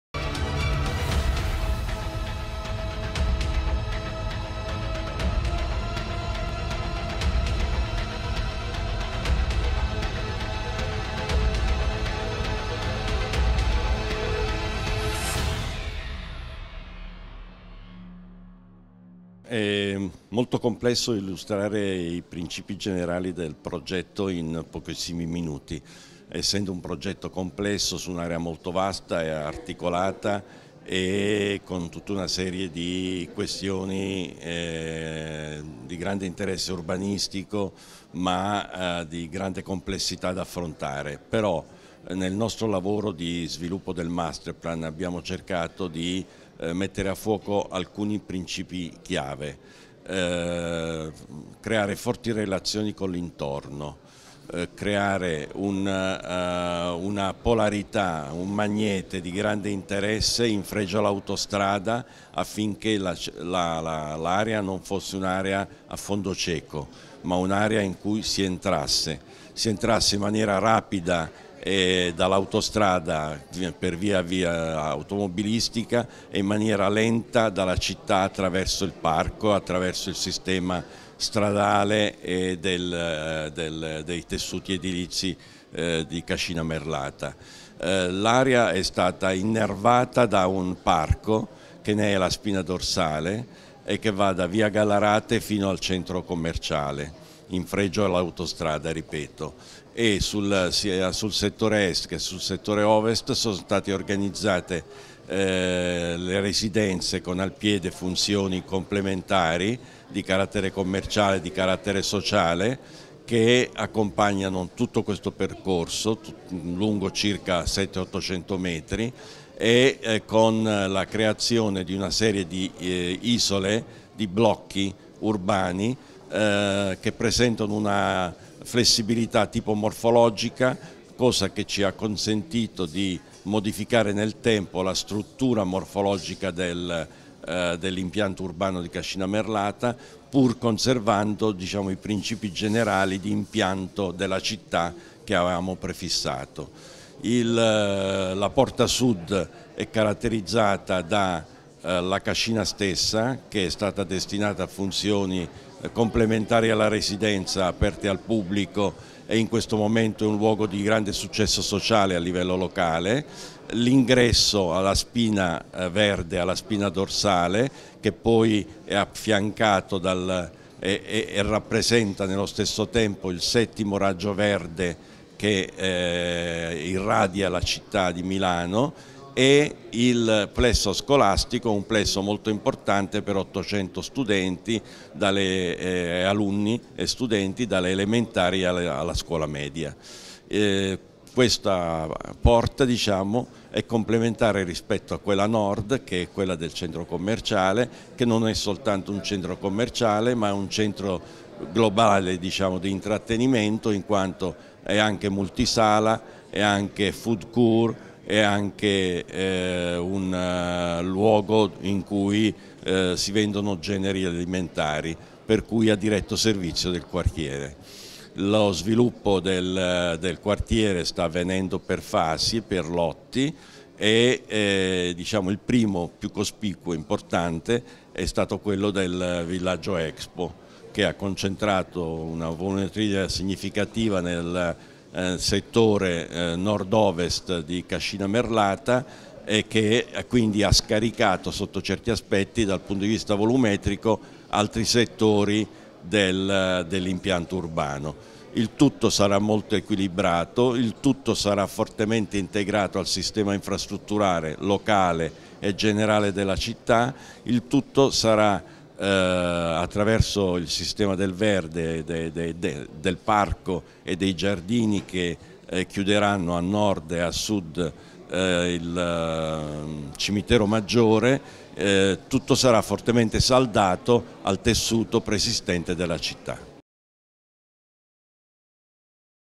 Passi in avanti per Cascina Merlata: Paolo Caputo (Caputo Partnership)
Intervista a Paolo Caputo di Caputo Partnership, in occasione dell'incontro "Dal masterplan di Cascina Merlata al quartiere Uptown"